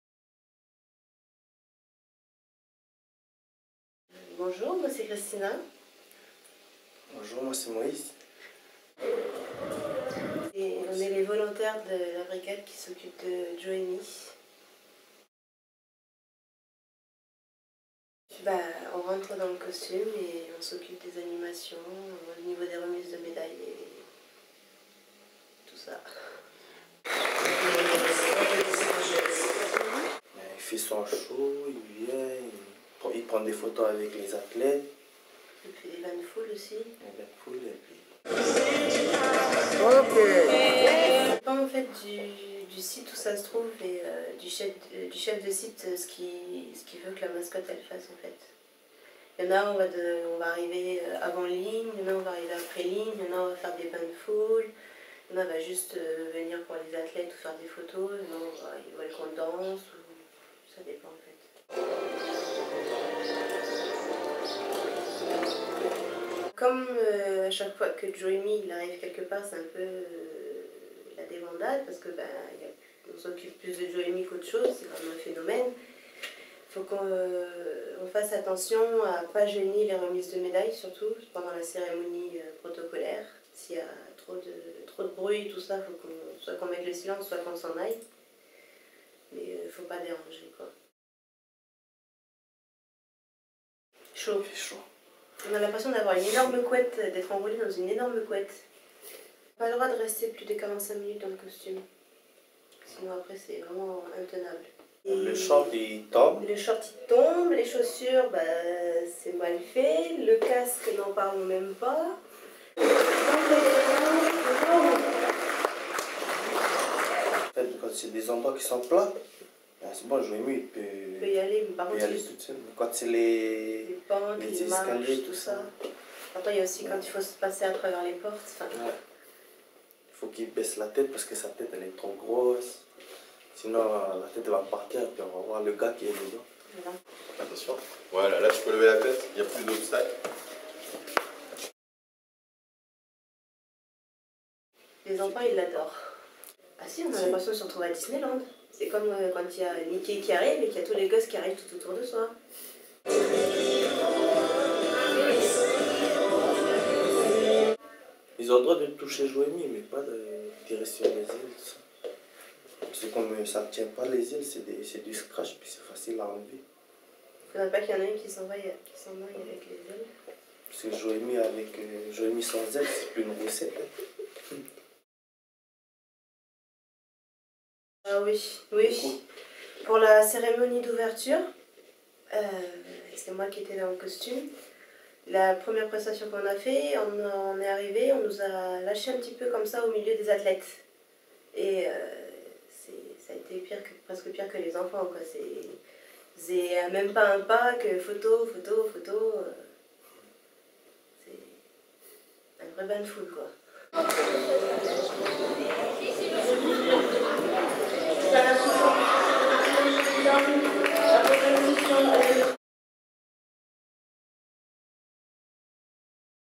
Ils font les Jeux : Moïse et Christina, « Joemy »
La mascotte des Jeux Joemy est partout. La roussette bleue participe aux animations et surtout à toutes les remises de médailles. Derrière le masque se cache une vingtaine d'étudiants de l'école de la 2e chance. Costume, popularité : Moïse et Christina nous raconte ce que ça fait d'être Joemy.
Une vidéo de LNC.NC, réalisée par Fabien Dubedout